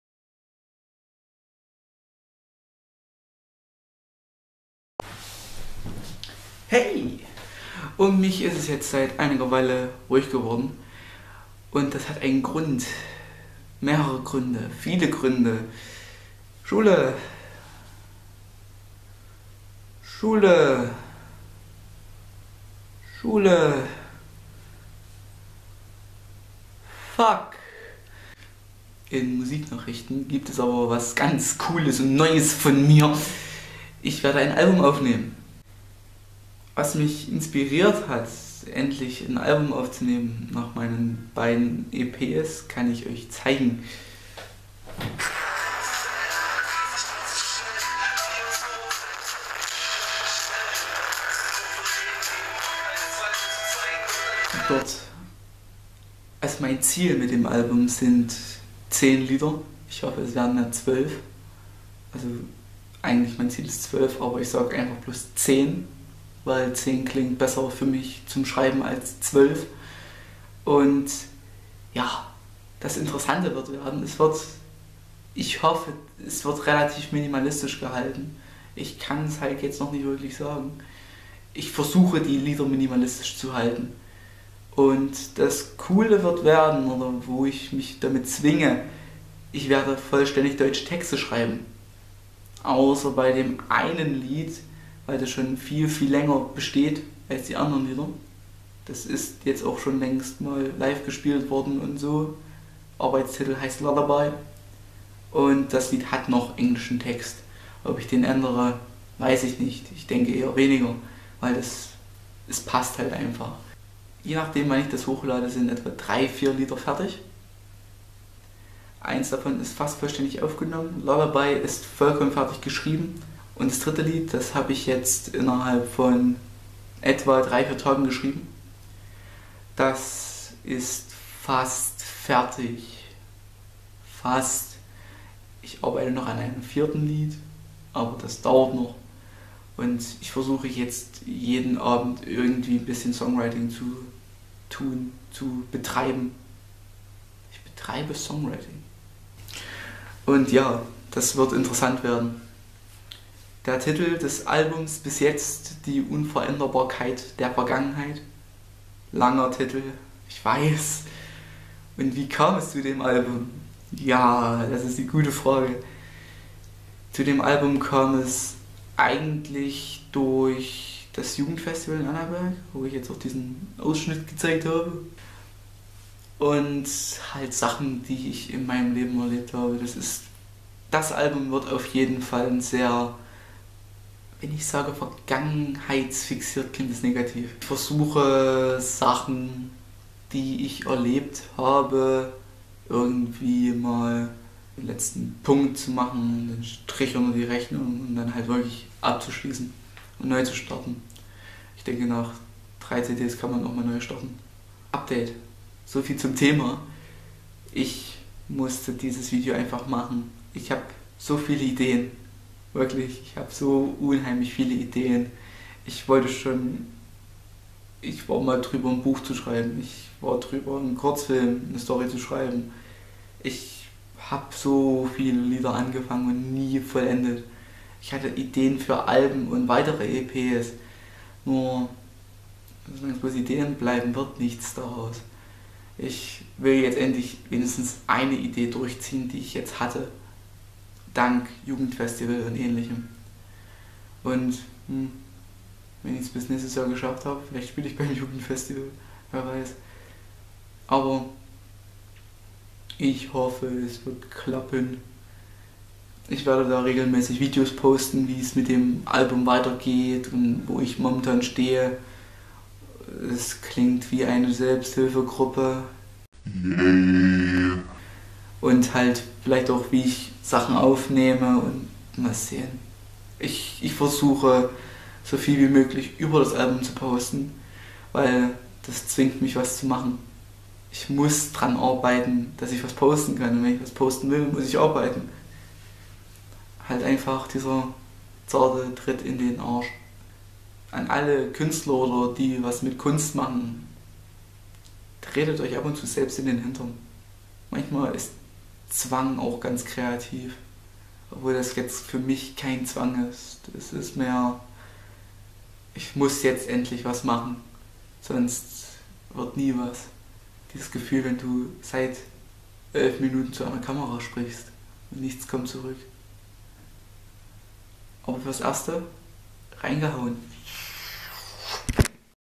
Vlog - Der Anfang (des Albums)
Endlich mal wieder ein Labber-Video
Es geht um das Album, die Gedanken dahinter und warum ich jetzt loslege damit.